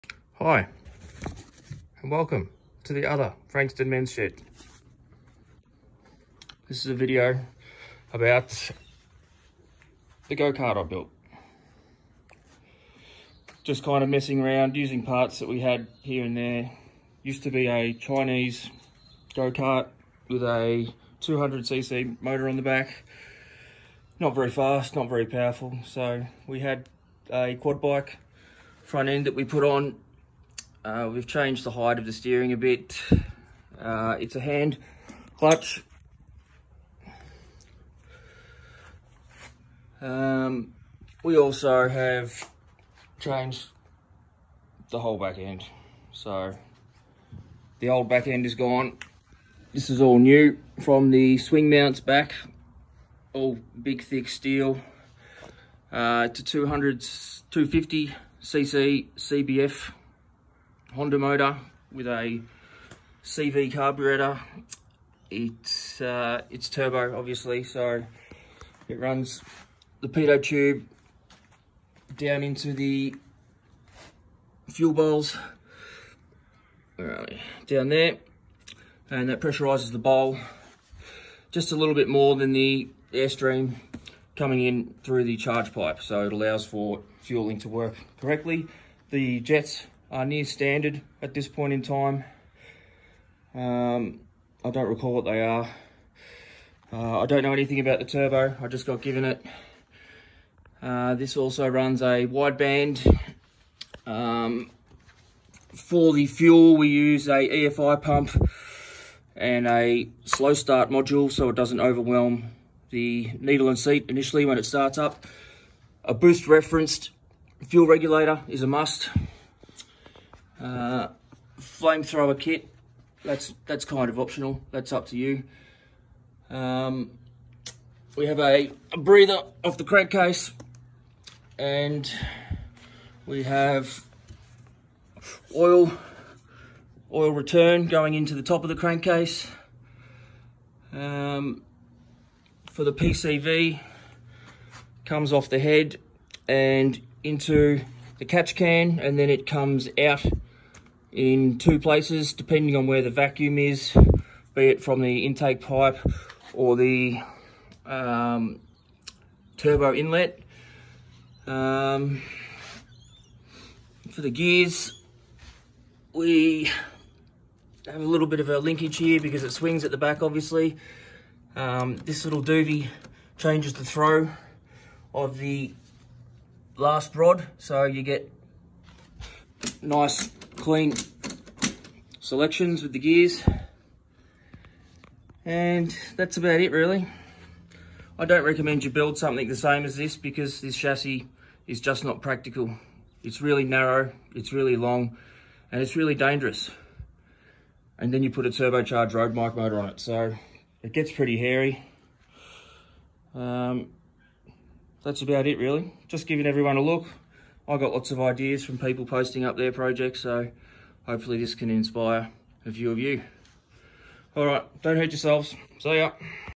250cc Turbo Go Kart
Howdy,
I had a lot of trouble finding recourses to build this project. It was my first time welding and retrofitting turbo. Btw I advanced the exhaust cam 1 tooth instead of changing head/bore gaskets.
It goes ok, needs more tuning though. It’s the wrong chassis for that much power and weight.
Peak boost has been 3 psi.

Cheers